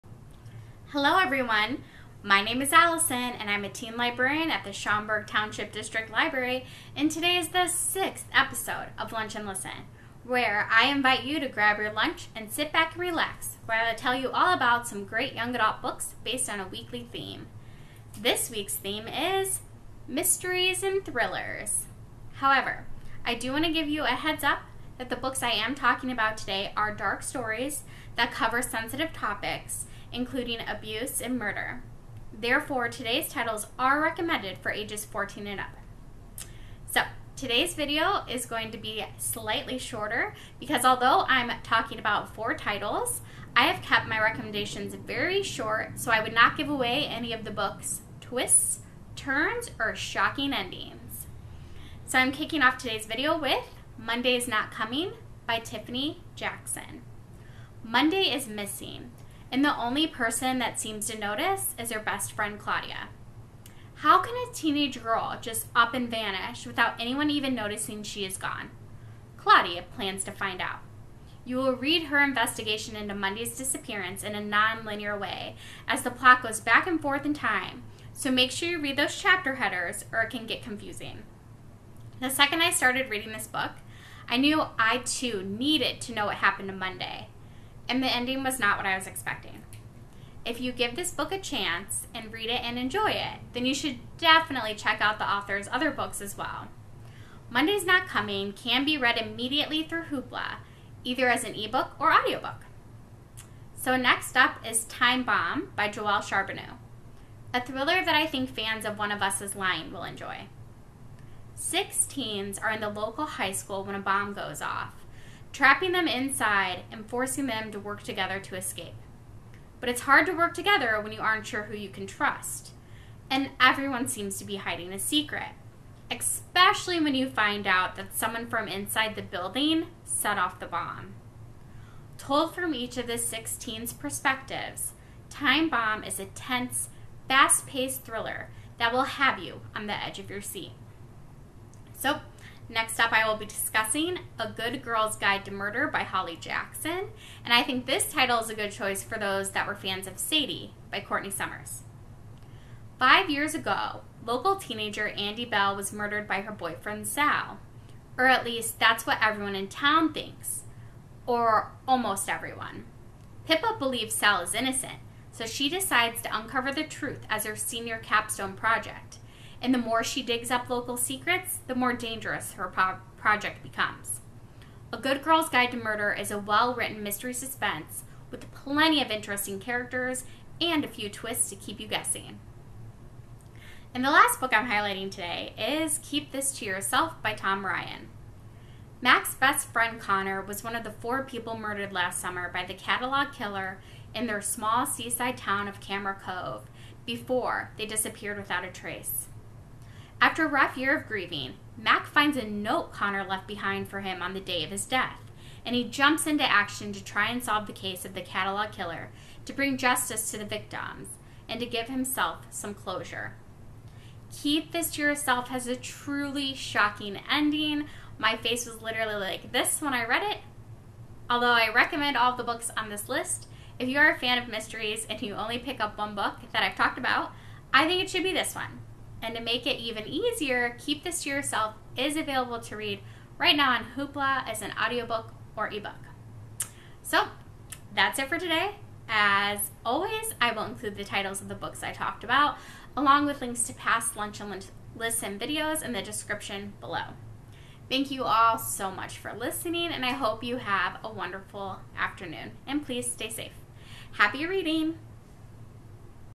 Lunch and Listen: Young Adult Book Recommendations
Join Teen Librarian Allison every other Tuesday for Young Adult book recommendations based on a specific theme. This Week: Mysteries and Thrillers

Books Mentioned:
Monday's Not Coming by Tiffany Jackson
Time Bomb by Joelle Charbonneau
A Good Girl's Guide to Murder by Holly Jackson
Keep This to Yourself by Tom Ryan